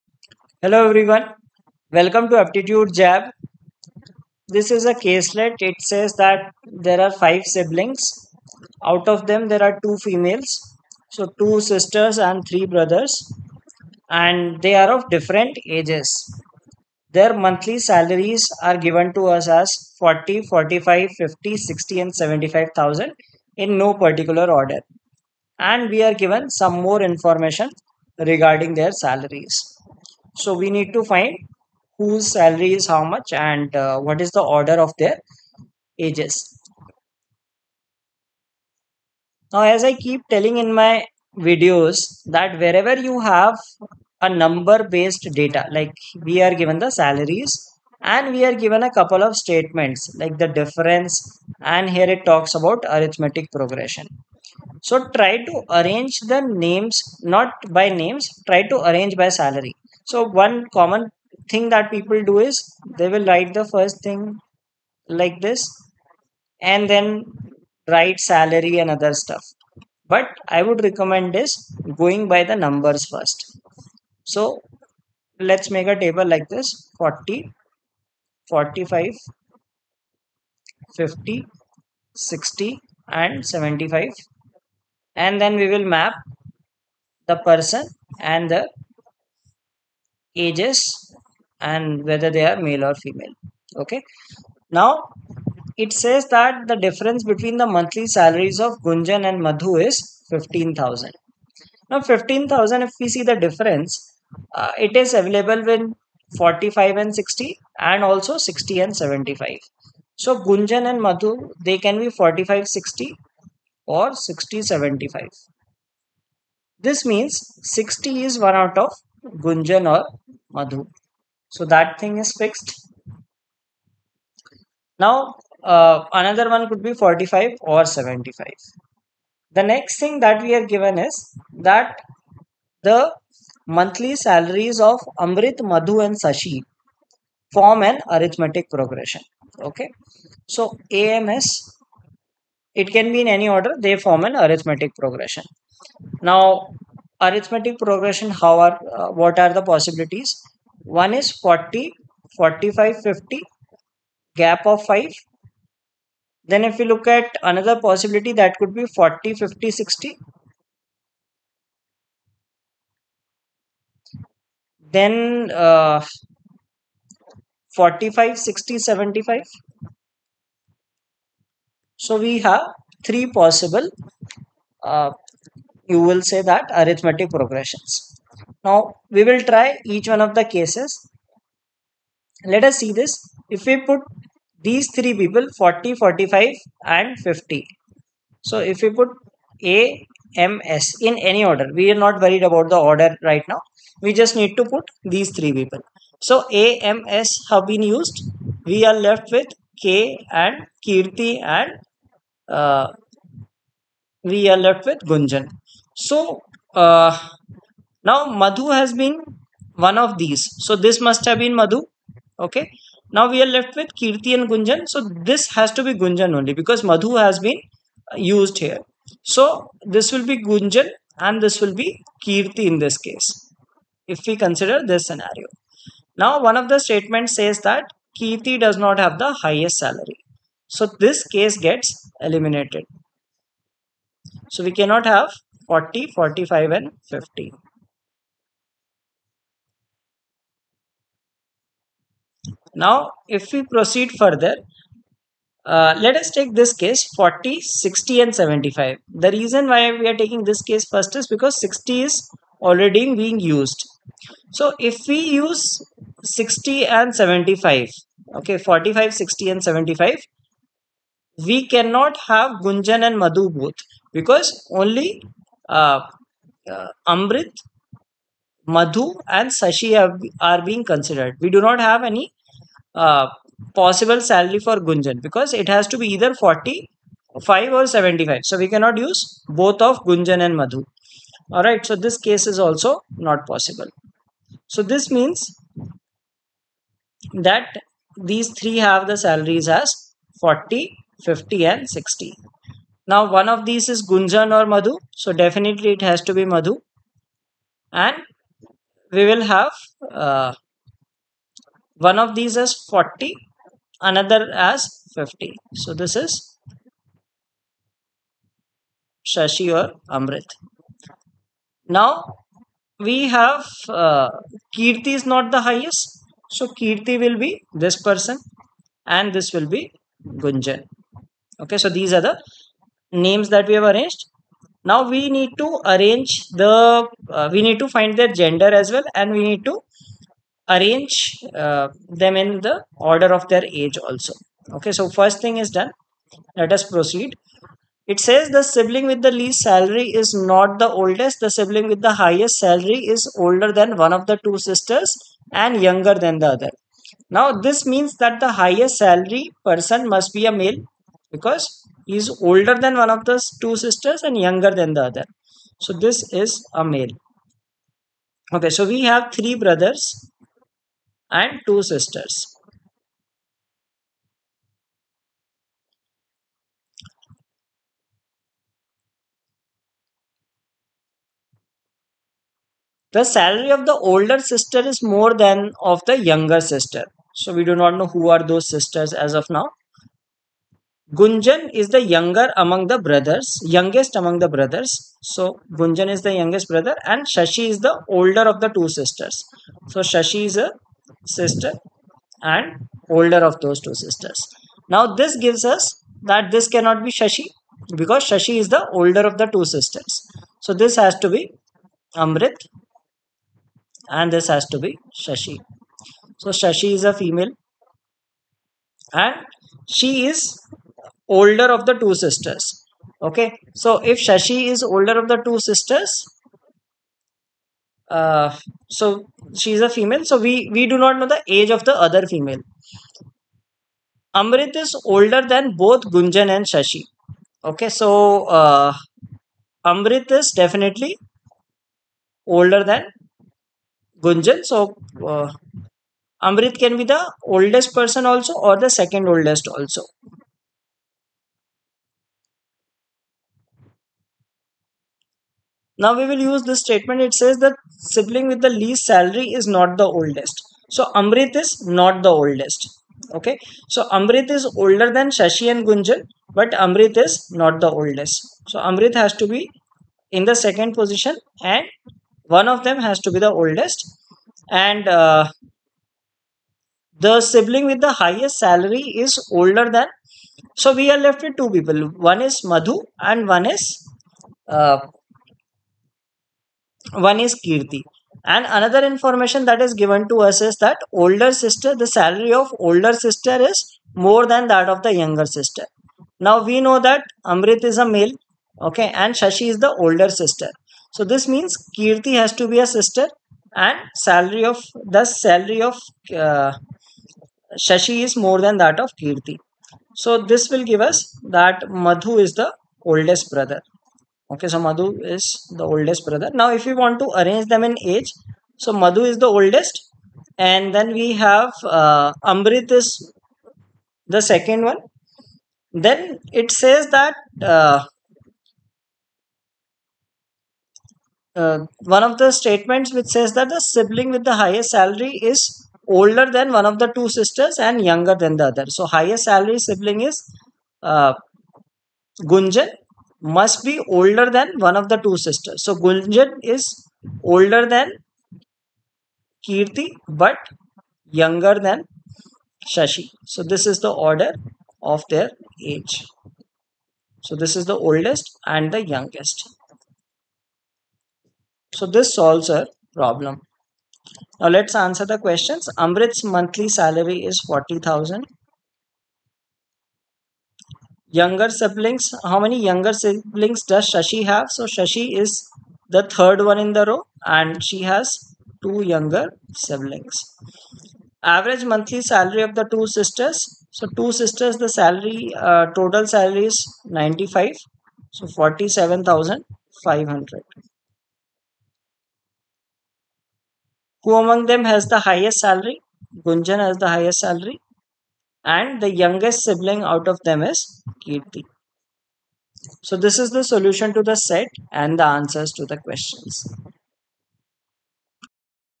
CAT Infinite DILR - Set 349 | Five Working Siblings | Caselet
Five siblings (three brothers and two sisters) have different salaries. Using the conditions find their salaries and ages.